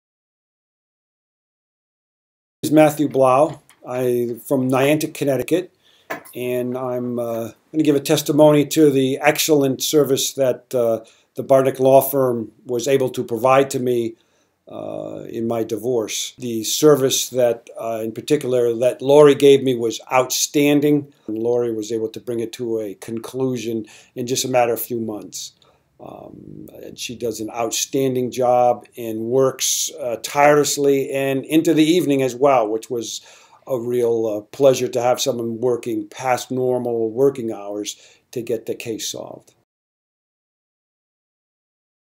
Client testimonial from Matthew Blau about Lori Bartinik attorney at law.
Client explains why he is happy with The Bartinik Law Firm.